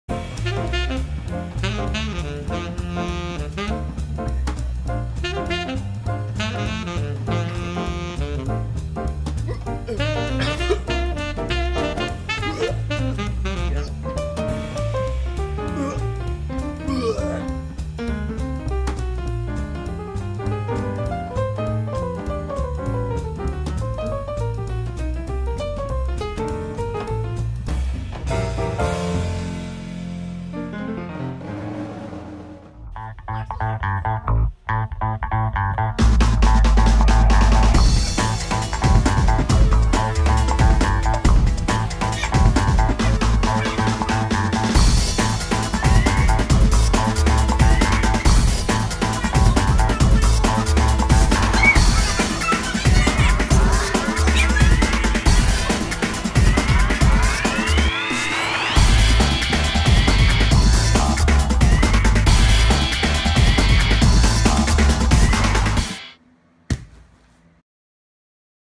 1st vs 3rd - Diaper Changing
Difference between diaper changing with your 1st and your 3rd